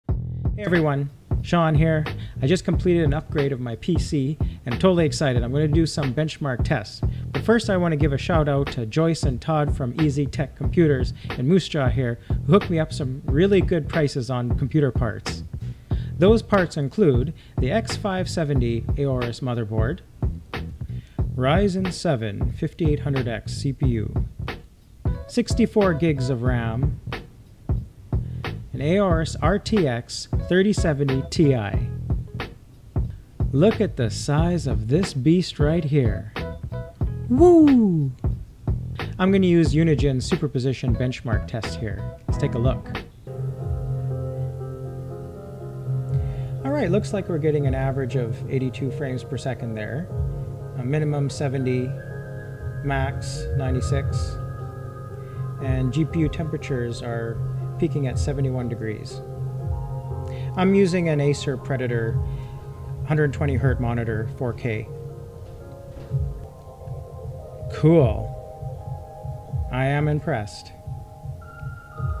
Benchmarking an Aorus RTX 3070 Ti
Unigine Superposition Benchmark test - Preset: 4K Optimized
Min FPS: 70; Avg FPS: 82; Max FPS: 96
Peak GPU Temperature at 71 Degrees Celsius.

The full specs of the PC are the following:
Gigabyte X570 Aorus Elite Motherboard
AMD Ryzen 7 5800X 8-core/16-thread CPU
Aorus Geforce RTX 3070 Ti
Corsair Vengeance 64 GB RAM DDR4
Cooler Master Masterliquid ML240R RGB CPU Cooler
Samsung 970 EVO 500GB M.2 SSD
Samsung 860 EVO 1TB SATA SSD
Corsair  AX1200 Gold PSU
Cooler Master HAF 932 Case
Acer Predator 27" 4K Monitor, 120 Hz (can be overclocked to 144 Hz)
(December 2021)
E-Z-TECH Computers special ordered the parts.